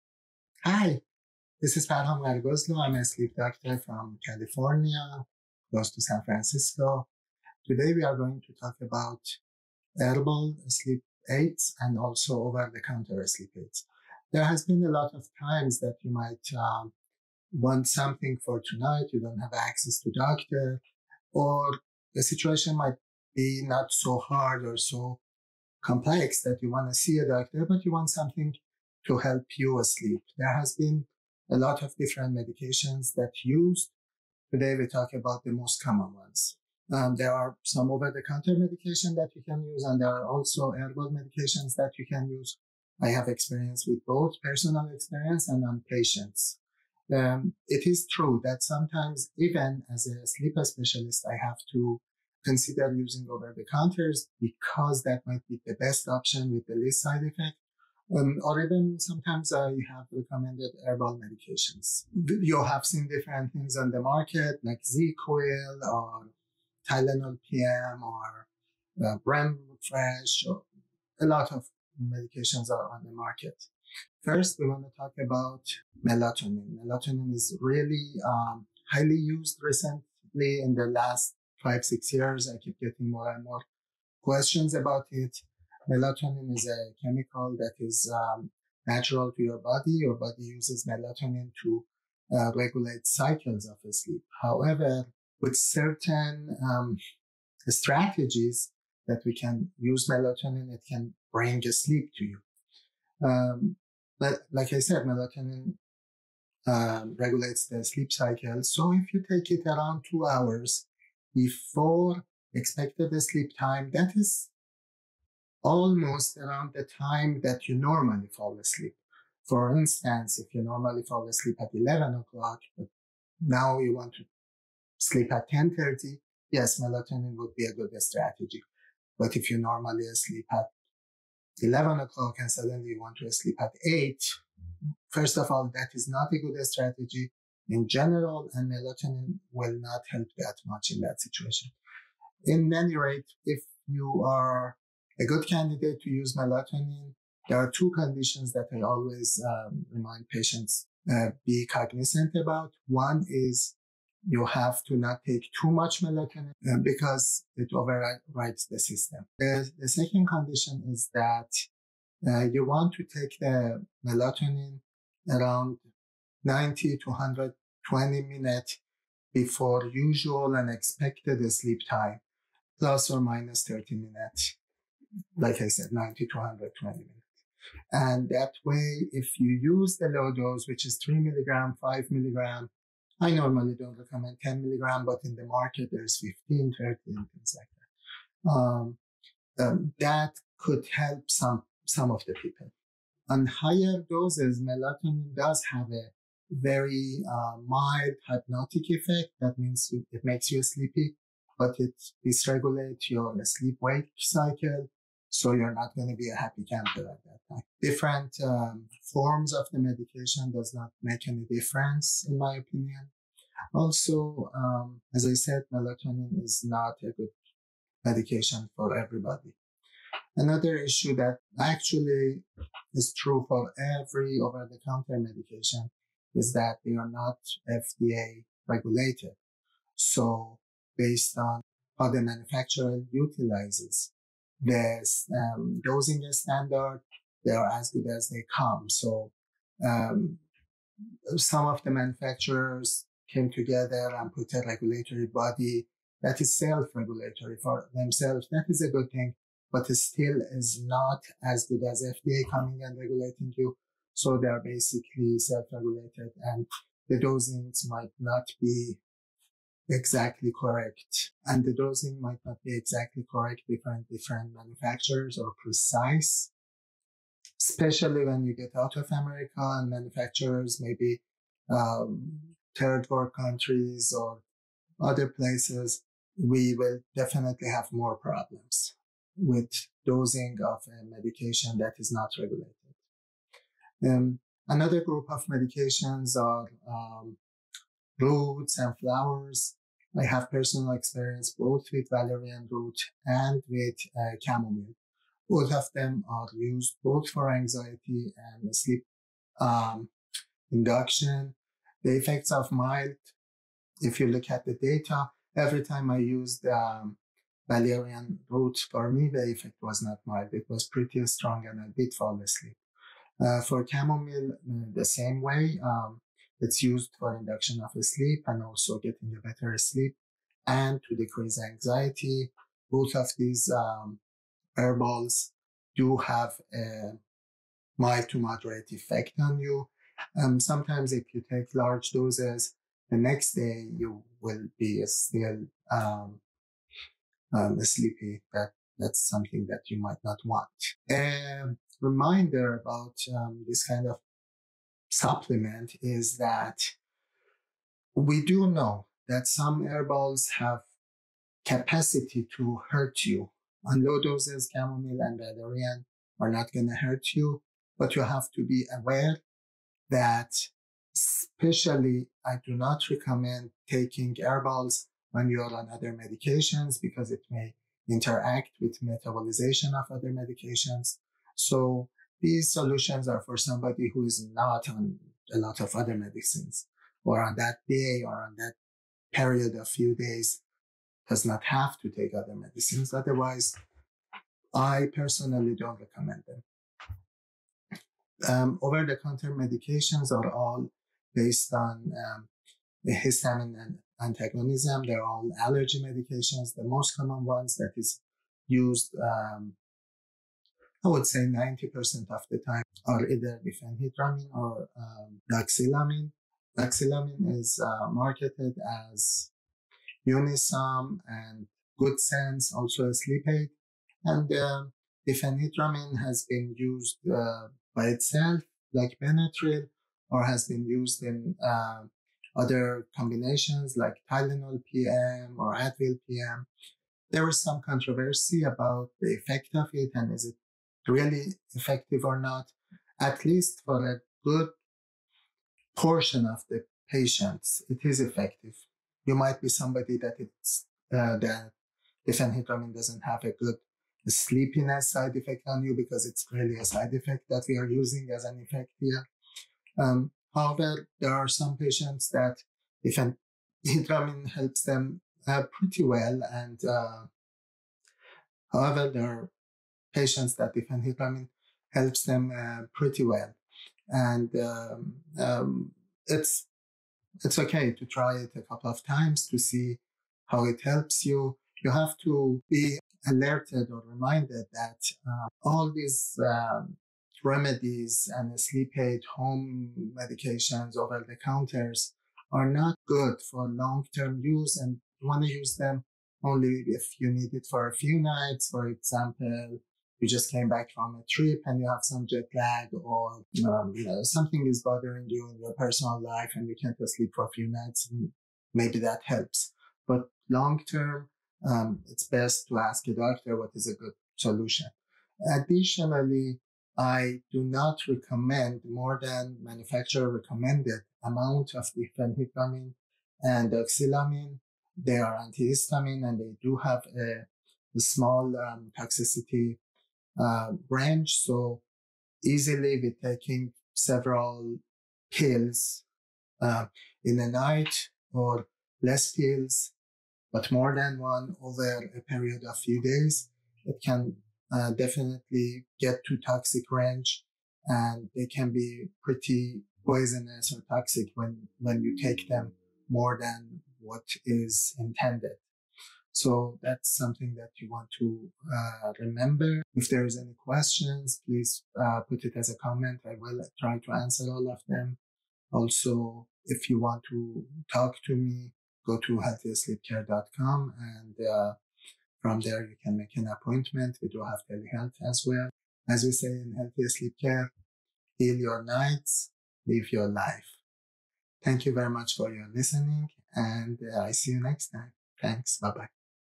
Insomnia aid without seeing a doctor: non-addictive drugs and herbals (sleep medications 2c)
Don't want to use hard drugs?

In today's video, we delve deep into the world of herbal and over-the-counter sleep aids, exploring their efficacy, safety, and mechanisms of action. As a certified sleep specialist, I break down the science behind popular options like melatonin, valerian root, and chamomile, giving you evidence-based insights to make informed choices for your sleep health. Whether you've considered incorporating these aids into your sleep routine or are simply curious about alternatives to prescription medications, this video provides a comprehensive yet easily digestible guide to help you catch those elusive Z's naturally and safely.

Art and editing done by Shayan Gharagozlou.

TIMESTAMPS
00:00 Introduction
01:15 Melatonin
04:50 Valerian and Chamomile
06:28 OTC Medication